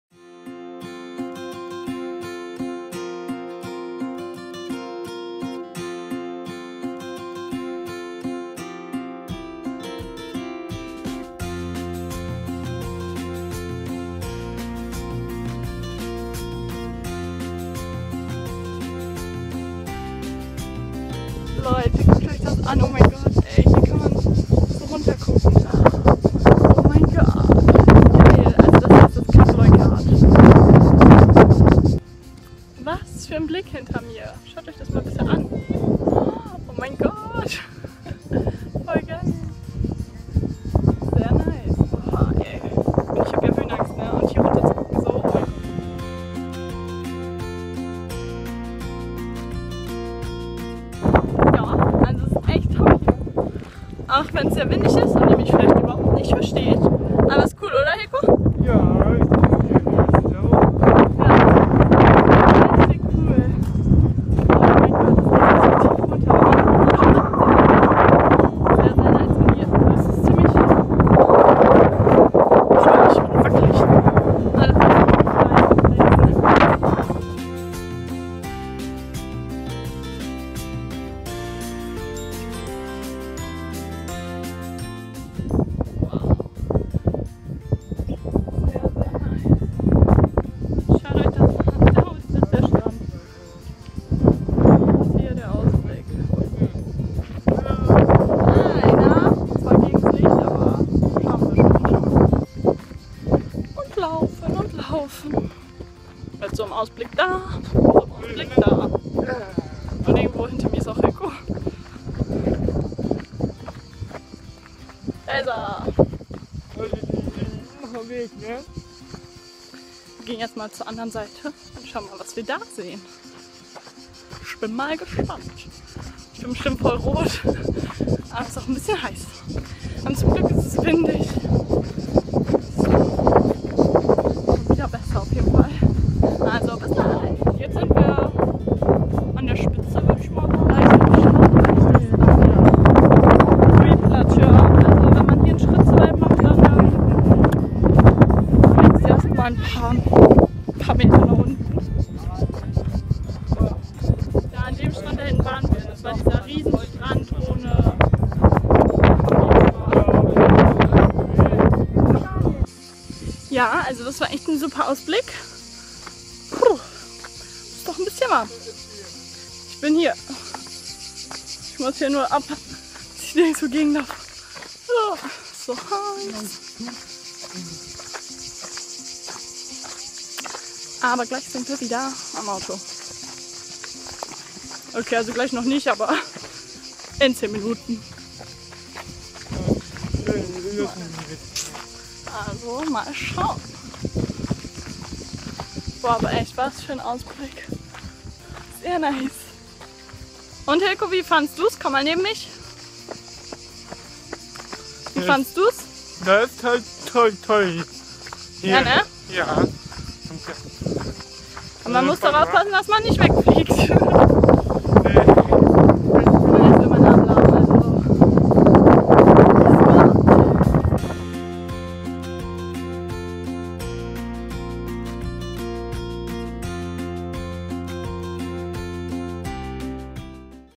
#5 Frankreich Tagebuch: Cap Leucate | talkingtomke
Heute hab ich für Euch auch schon das Video aus unserem Urlaub. Wir waren am Cap Leucate. Es war da oben echt sehr windig, also bitte entschuldigt das Rauschen des Windes im Video...✌️:)

Ich würde mich sehr freuen :)

Eure Tomke 💖